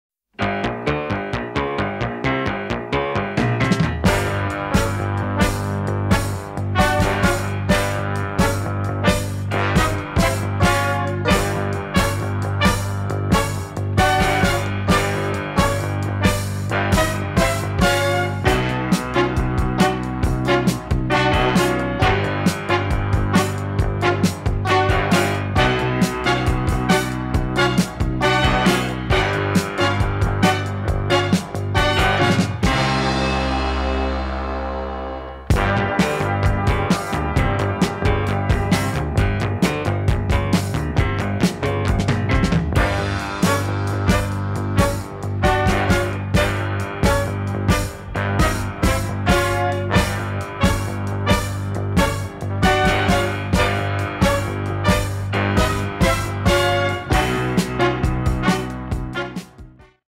N Gauge layout.
All layout's showing are one's i build for other people, this one was to be redone and then 6 month's later made bigger.